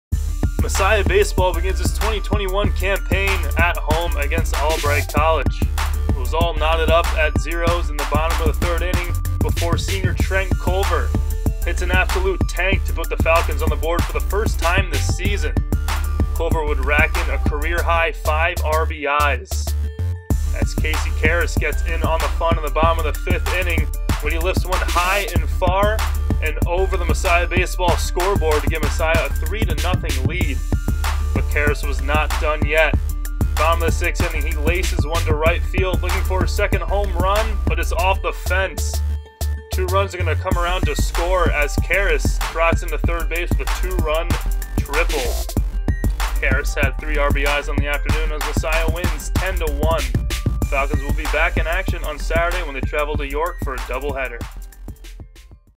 Messiah Baseball Highlight of the Night - March 9, 2021
Messiah Baseball opened its 2021 season at home with a dominant 10-1 victory over Albright College! Trent Culver (5 RBIs) and Casey Karas (3 RBIs combined to knock in eight of the Falcons' 10 runs!